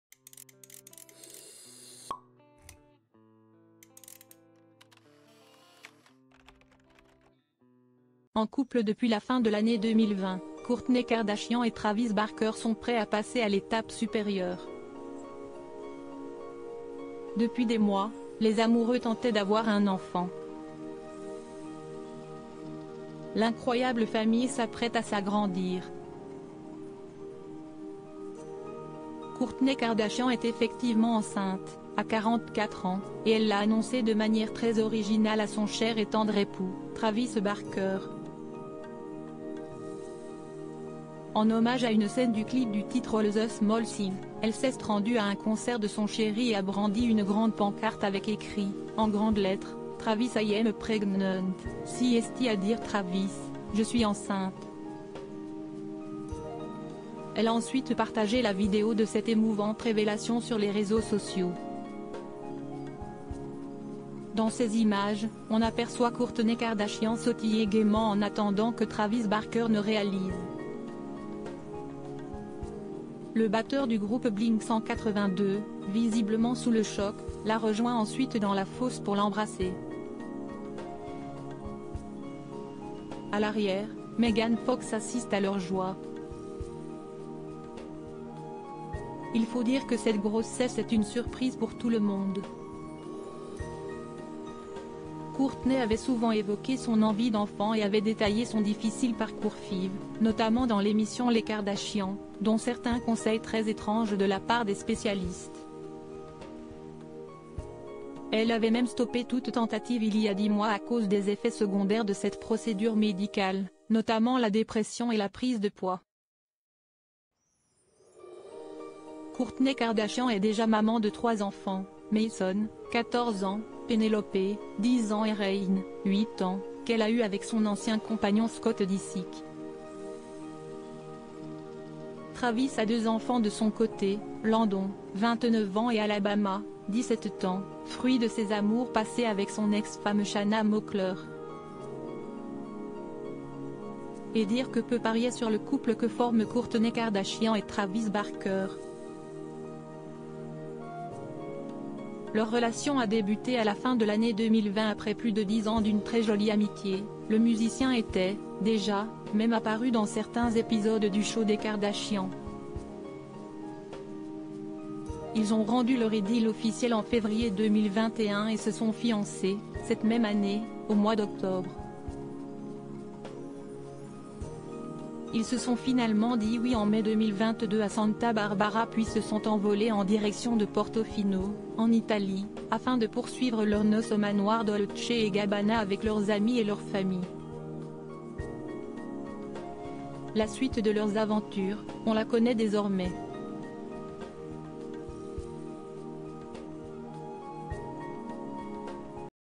VIDEO Kourtney Kardashian enceinte à 44 ans, l'émouvante révélation filmée en plein concert de Travi
VIDEO Kourtney Kardashian enceinte à 44 ans, l'émouvante révélation filmée en plein concert de Travis Barker !